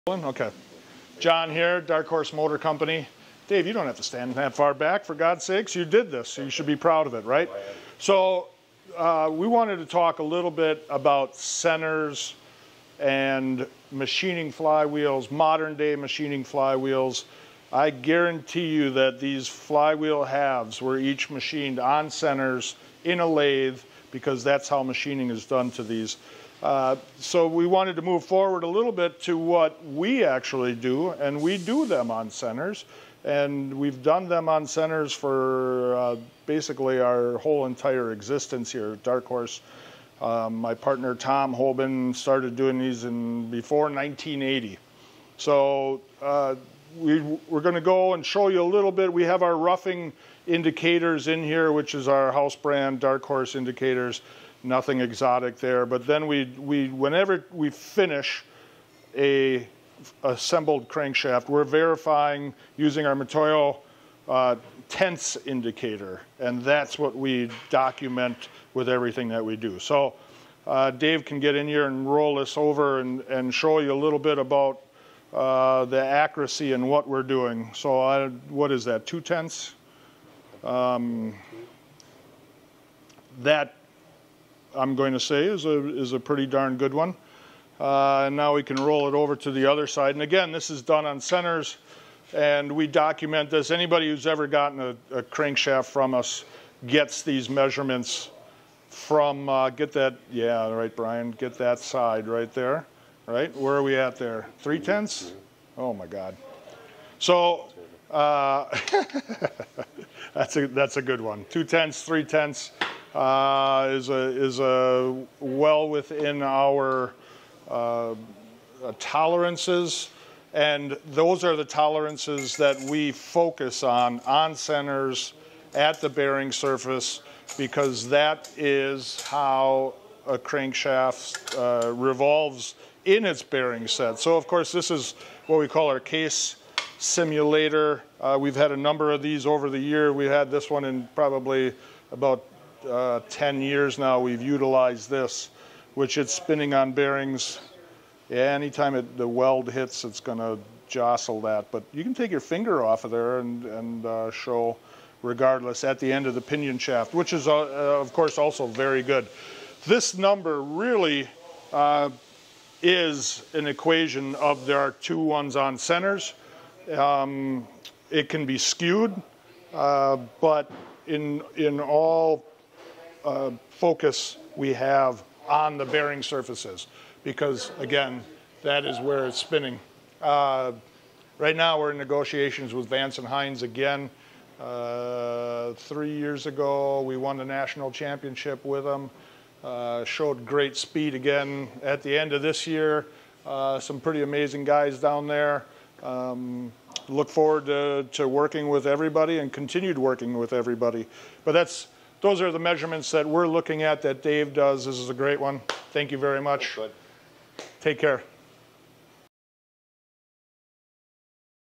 Darkhorse Defends Centers pt.1
John and Dave from Darkhorse Moco talk about why we measure Harley-Davidson crankshafts on machinist centers.